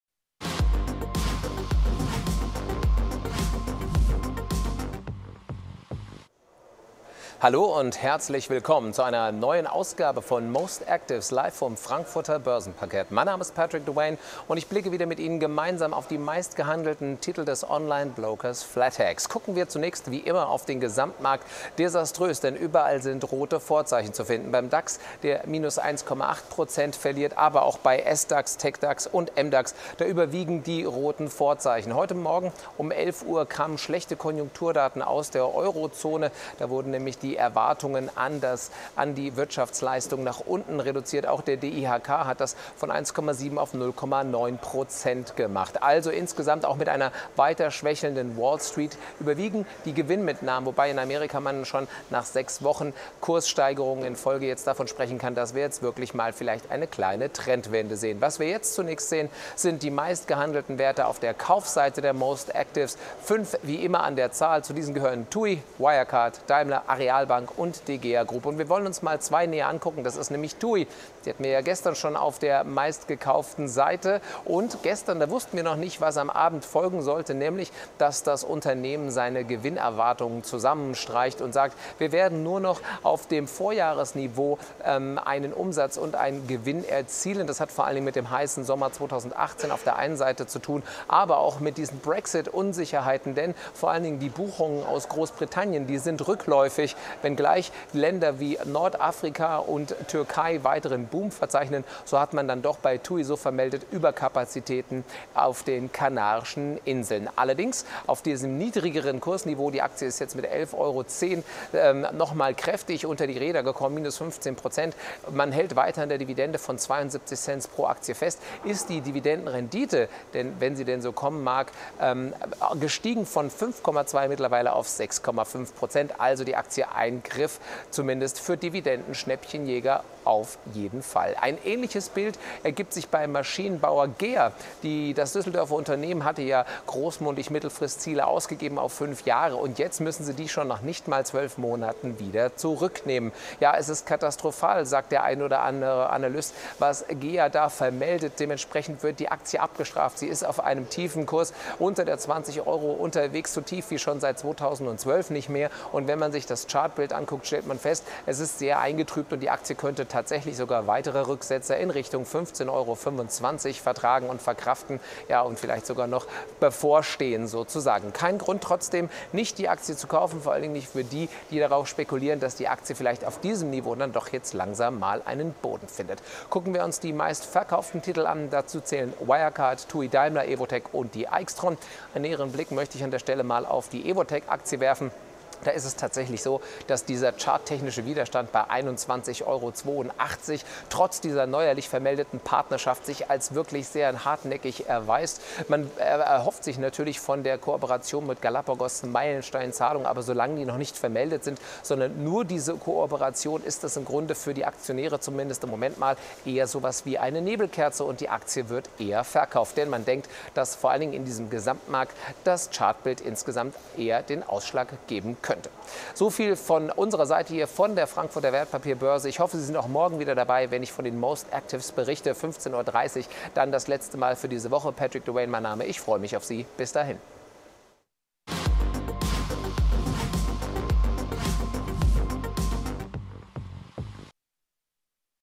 Most Actives: Prognosesenkungen und neue Partnerschaft
Patrick Dewayne blickt auf die meistgehandelten Werte beim Onlinebroker Flatex. Welche Aktien werden aktuell am häufigsten gekauft und welche verkauft. Zudem erfahren Sie, warum einige dieser Werte heute im Fokus stehen.

Unter den Käufen finden sich heute zwei Konzerne, die ihre Prognosen gesenkt haben: TUI und die Gea Group. Anleger sind offenbar auf Schnäppchenjagd.

Bei den Verkäufen steht Evotec in Fokus. Der Konzern hat eine neue Partnerschaft bekanntgegeben und erhält dafür eine Vorabzahlung.